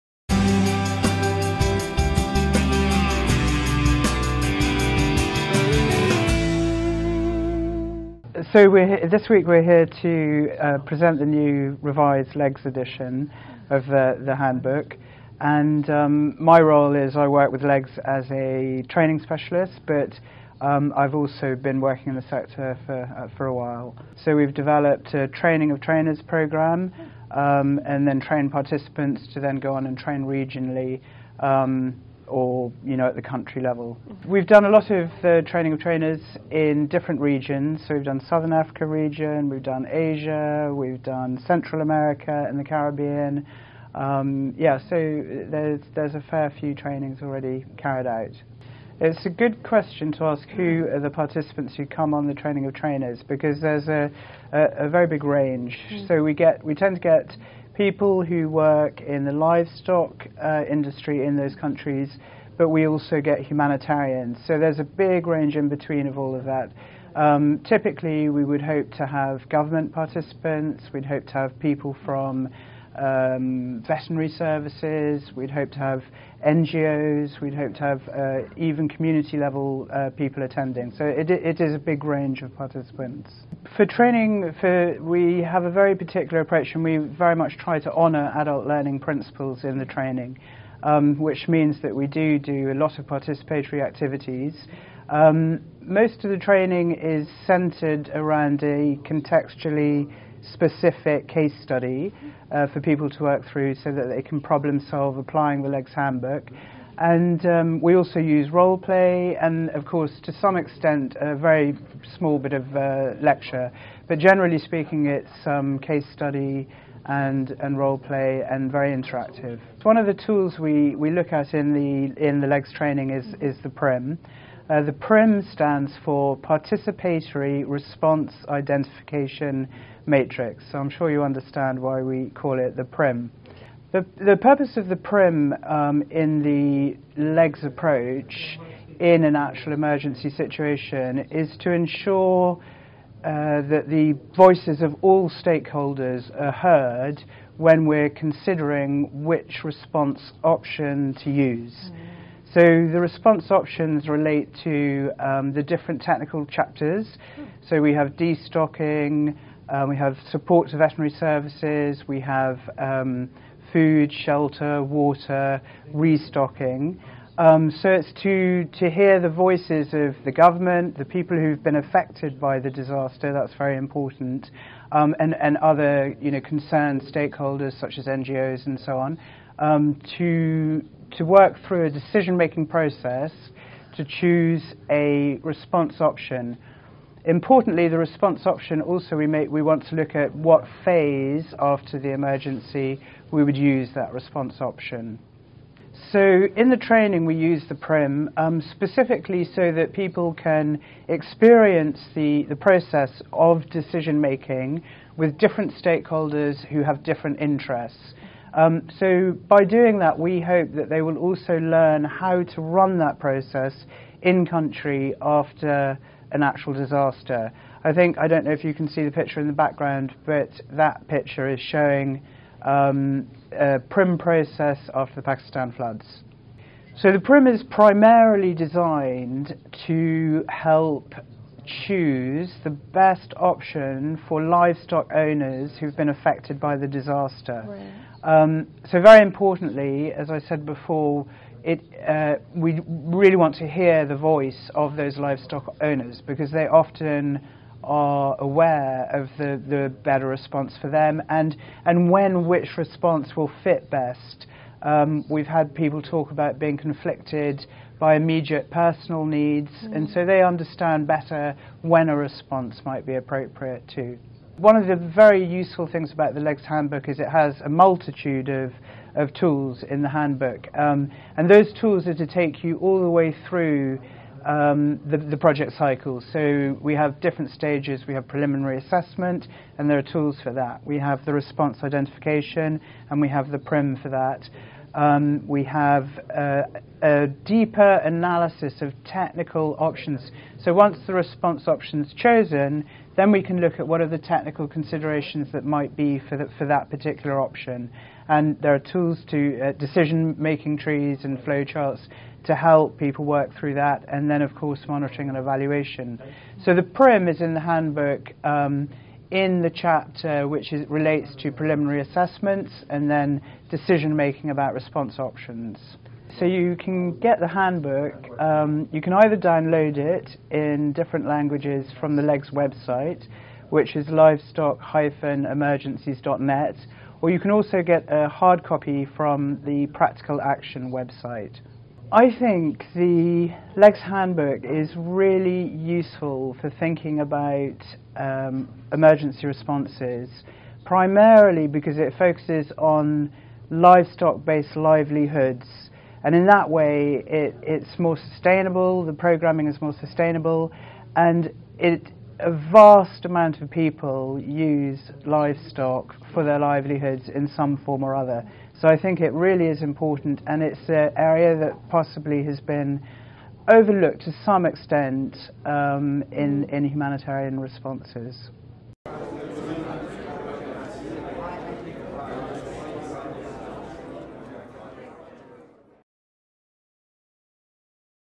Participatory Response Identification Matrix (PRIM)
In an effort to minimize unintended consequences and build upon existing market capacity, tools like the Participatory Response Identification Matrix (PRIM)—found in the new LEGS manual—can be used to facilitate discussion with different stakeholders and identify interventions that are most appropriate, given the context of the initial assessment. Because the results of the participatory discussion reflect the viewpoints of stakeholders, this can ultimately guide decision-making and minimize unintended consequences.